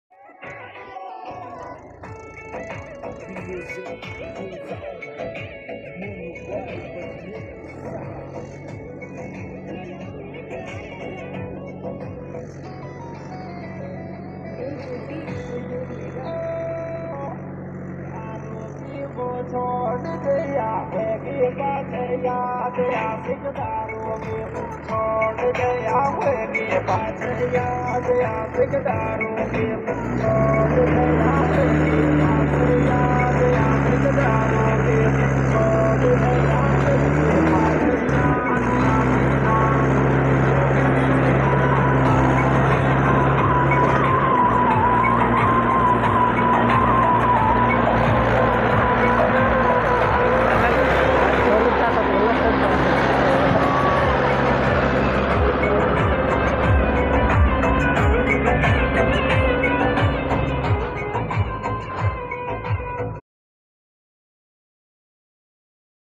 बजरी माफिया की बजरी से भरी ट्रॉली पलटने से बाल-बाल बची
बजरी माफिया के बजरी की ट्रॉली पलटने से बाल-बाल बचे ।
इतनी ऊंची चढ़ाई में ट्रॉली कितनी टेडी होकर निकलती है।      स्टैंडर्ड G.L. बजरी माफिया।।